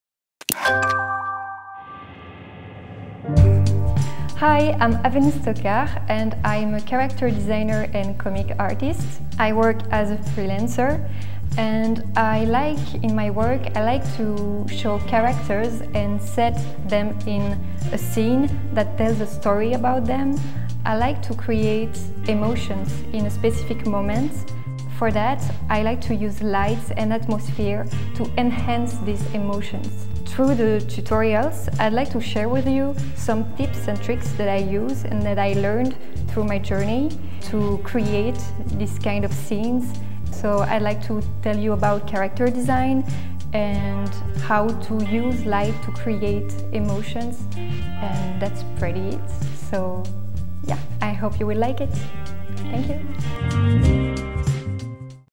Online Course: Cinematic style illustration by Aveline Stockart.
During this tutorial with Aveline Stokart you will learn how to create an atmosphere illustration from beginning to end. You will learn how to select pictures, the adequate composition of the ambience and the characters.

Their personality, how to make direct and diffuse lights, how to balance the shades and till the perfect resolution for your illustration.

Also, you will see how to define lines or how to color, balance the shades and blur, how to complete the skin and hair texture or the face, eyes,nose, and lips details.

Overall will give our illustration a film taken look.